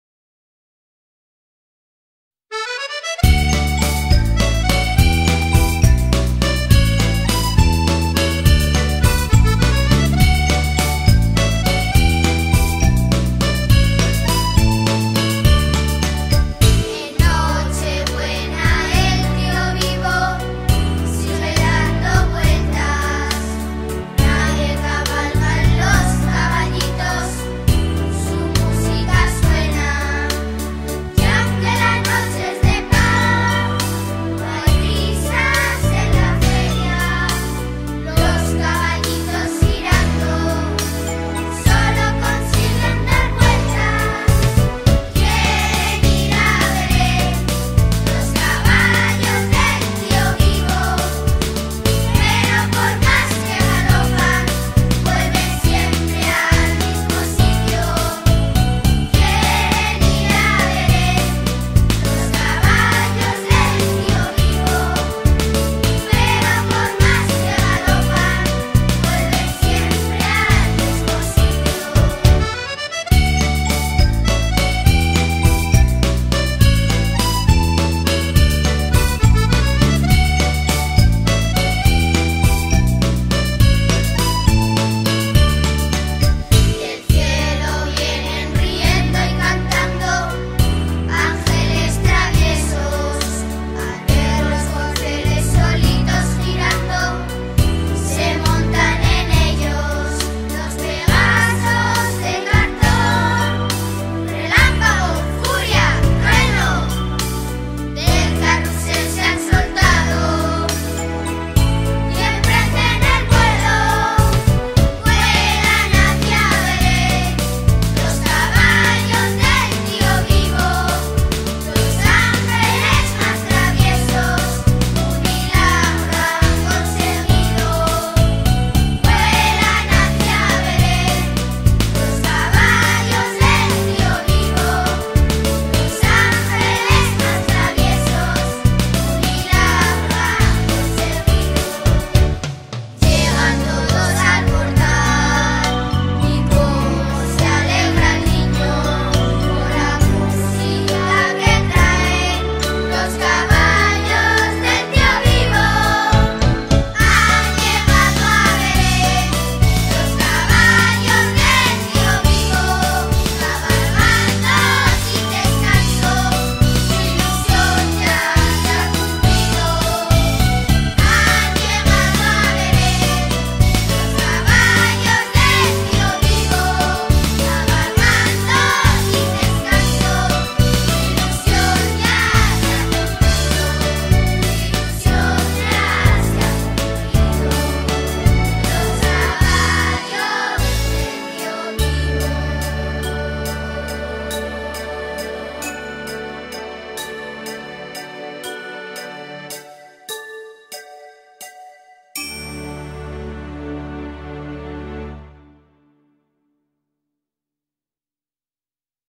LOS CABALLOS DEL TIOVIVO
Los caballos del Tiovivo son capaces de salir de su rueda, que les lleva una y otra vez al mismo sitio, para acercarse hasta Belén, calentar el establo y postrarse ante el Niño. Y eso es lo que más alegra al Él: saber que su llegada es capaz de cambiar vidas, sacarnos de rutinas y descarrilar de ruedas que -por más que cabalgues- no te llevan a ningún sitio.